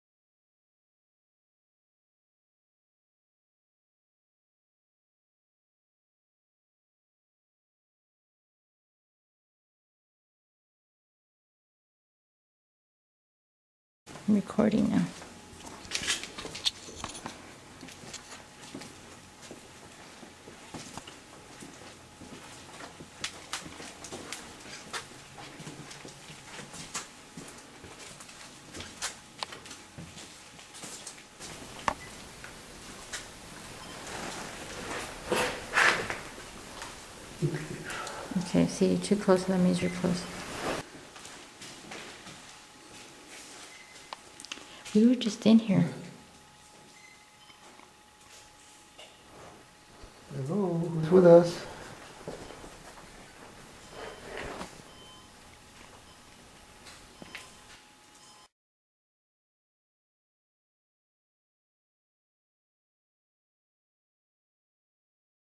Old Yoakum Hospital EVPs
These EVPs were just a few that were taken on the first floor of the hospital.